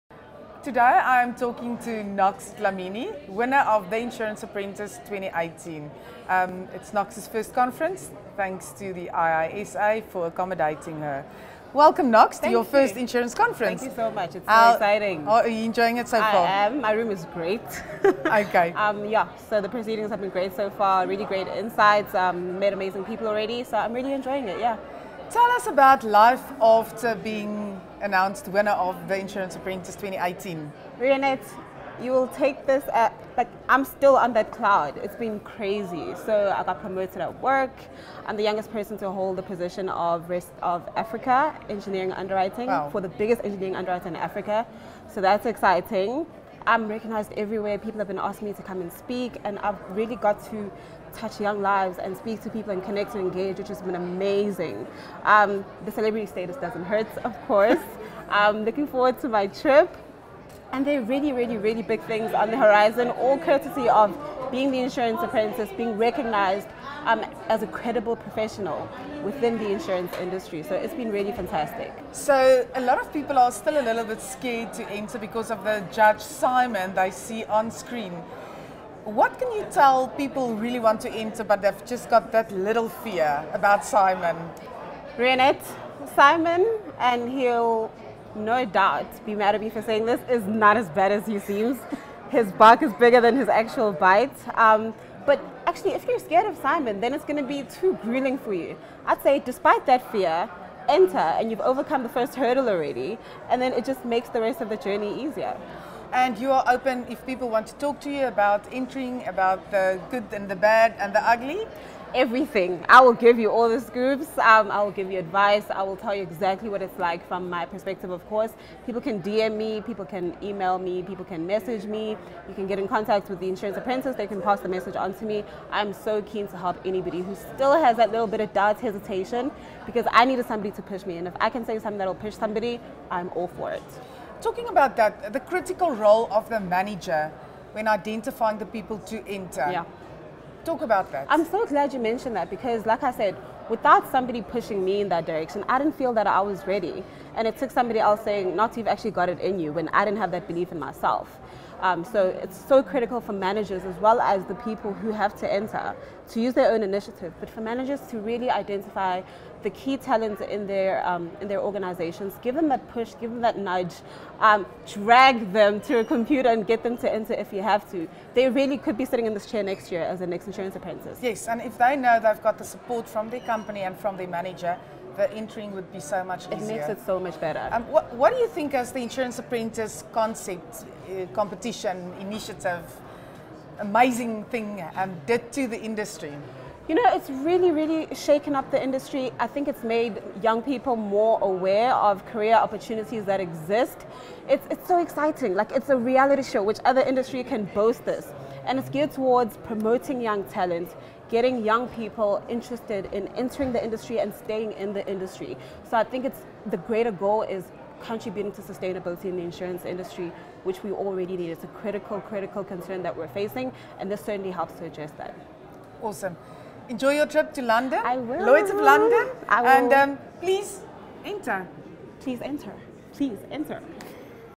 FAnews interviews Noxolo Dlamini, winner of The Insurance Apprentice 2018
FAnews caught up with Noxolo Dlamini, winner of The Insurance Apprentice 2018, at the 2018 Insurance Conference at Sun City. Nox spoke about life after The Insurance Apprentice.